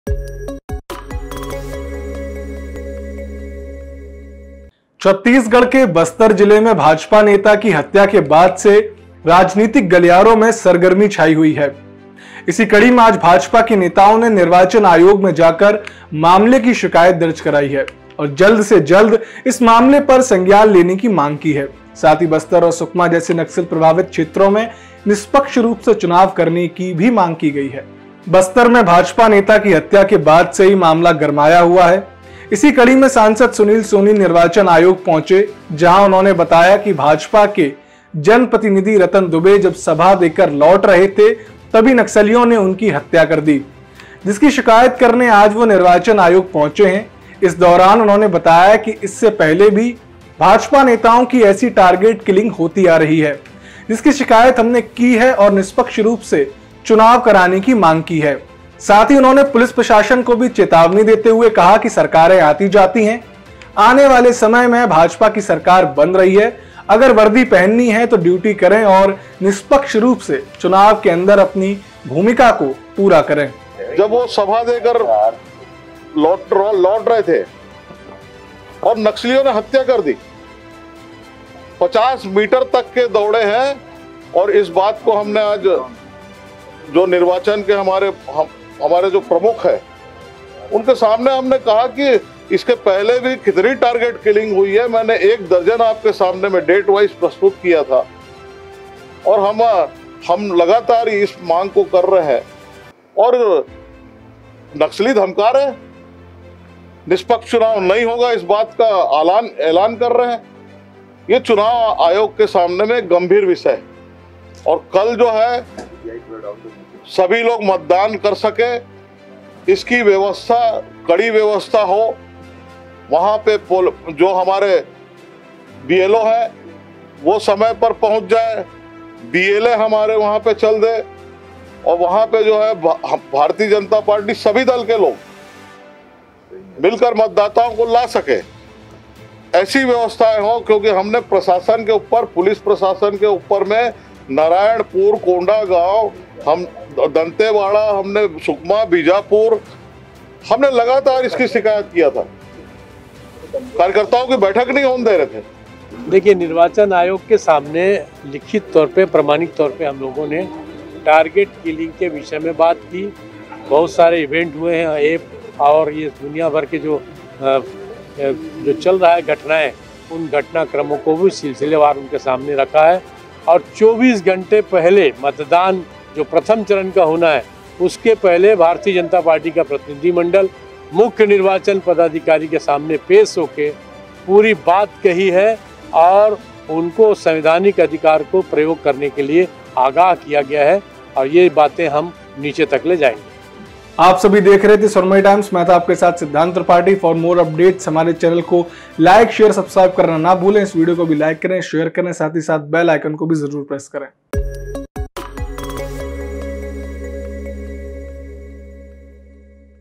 CG News : BJP नेता की हत्या से राजनीतिक गलियारोंमें छाई सरगर्मी
छत्तीसगढ़ के बस्तर जिले में भाजपा नेता की हत्या के बाद से राजनीतिक गलियारों में सरगर्मी छाई हुई है। इसी कड़ी में आज भाजपा के नेताओं ने निर्वाचन आयोग में जाकर मामले की शिकायत दर्ज कराई है और जल्द से जल्द इस मामले पर संज्ञान लेने की मांग की है । साथ ही बस्तर और सुकमा जैसे नक्सल प्रभावित क्षेत्रो में निष्पक्ष रूप से चुनाव करने की भी मांग की है.

Swarmayi Times